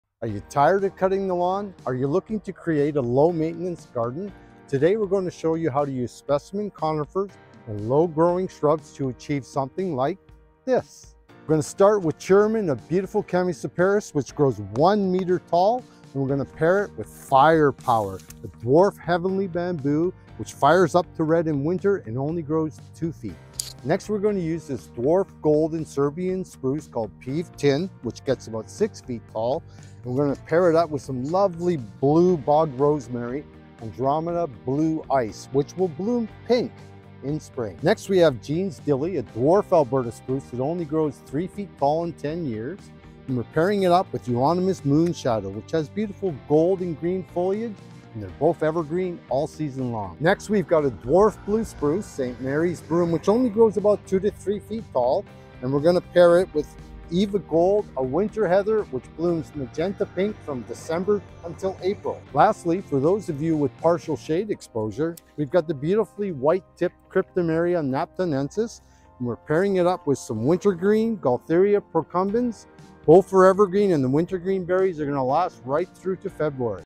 Perfect Pairings: Conifers & Foliage Shrubs for Easy-Care Gardens
Looking to create a low-maintenance yet colourful garden? Design a stunning, easy-care space with a mix of specimen conifers and complementary foliage shrubs!

Here are five beautiful plant pairings to inspire your garden:

✨ Chamaecyparis obtusa 'Chirimen' (Z5) & Nandina 'Firepower' (Z6)
✨ Picea omorika 'Peve Tijn' (Z4) & Andromeda 'Blue Ice' (Z2)
✨ Picea glauca 'Jean’s Dilly' (Z4) & Euonymus 'Moonshadow' (Z4)
✨ Picea pungens 'St. Mary's Broom' (Z2) & Erica x darleyensis 'Eva Gold' (Z6)
✨ Cryptomeria 'Knaptonensis' (Z5) & Gaultheria procumbens (Z3)

💾 Save this post for quick future reference and be sure to follow for more plant-pairing inspiration!